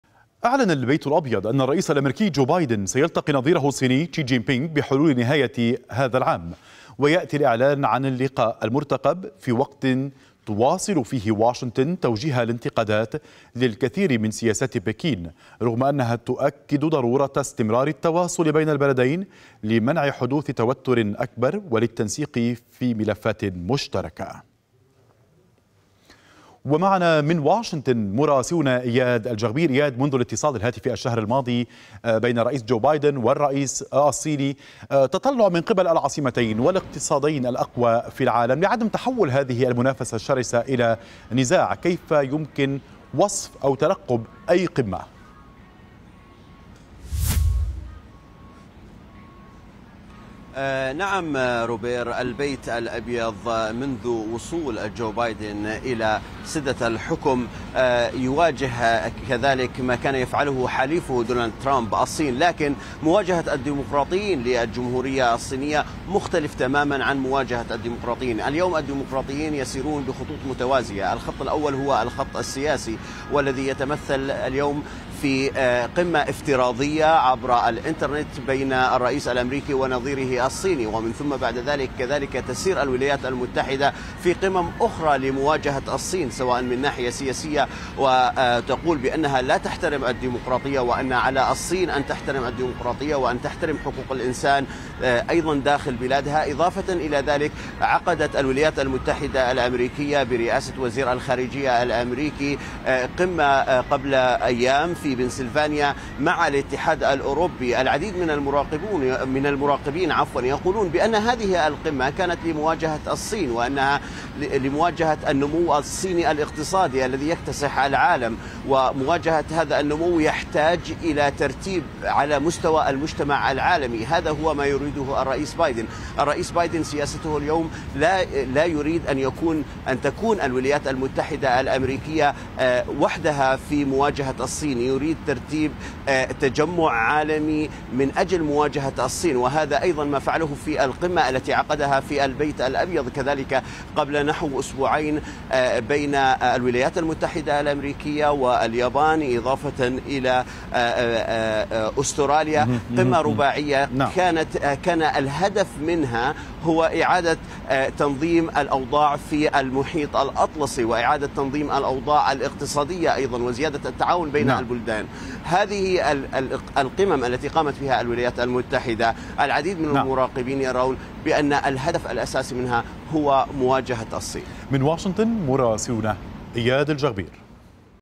واشنطن ترفض وصف علاقتها مع الصين بأنها صراع
أعلن البيت الأبيض أن الرئيس الأميركي جو بايدن سيلتقي نظيره الصيني شي جين بينغ بحلول نهاية العام الجاري.
ويأتي الإعلان عن هذا اللقاء المرتقب في الوقت الذي تواصل فيه واشنطن توجيه الانتقادات للكثير من سياسيات بكين رغم أنها تؤكد على ضرورة استمرار التواصل بين البلدين لمنع حدوث توتر أكبر، وللتنسيق في ملفات ذات اهتمام مشترك..